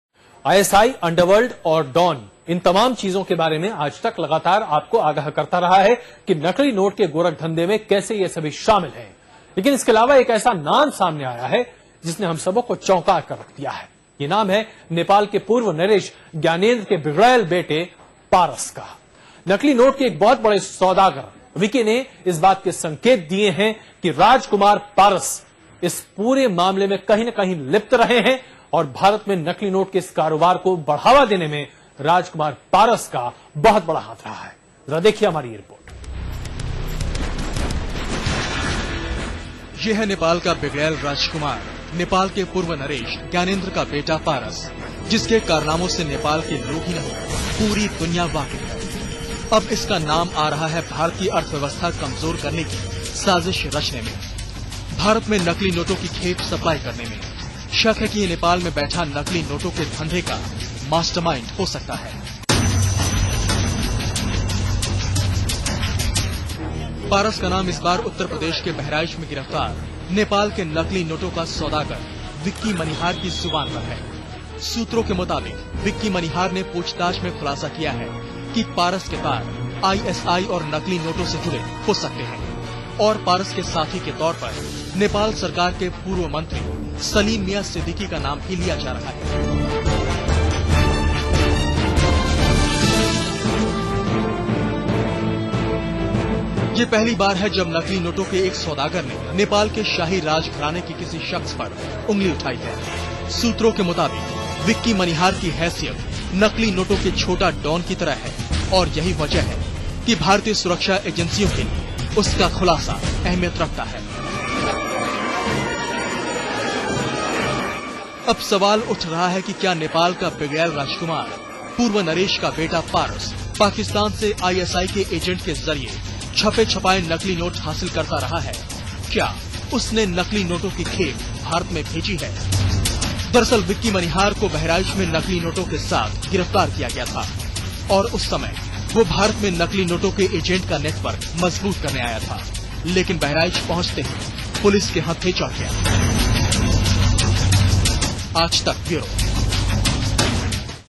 Nepal royals in Indian fake notes racket?
Does fake currency routed into India by Pakistan via Nepal have deep connections with Kathmandus royalty? Yes, according to Indian intelligence officers.